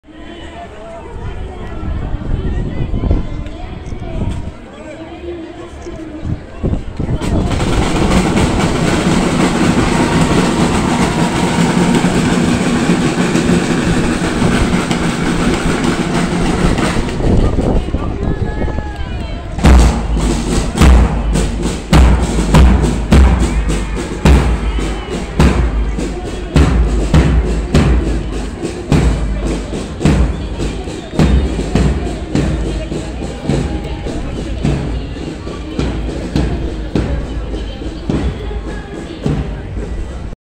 ブルガリアの旅 カザンラクの薔薇祭りパレード
ブルガリアへ旅をしました。
音楽隊の皆さんと音に合わせて行進され、華やかなパレードでした